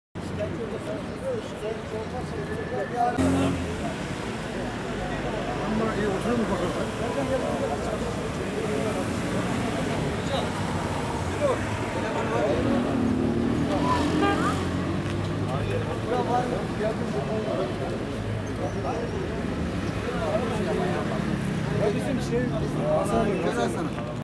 Otomobil durağa Girdi
Çankırı da Kaza araç otobüs durağına girdi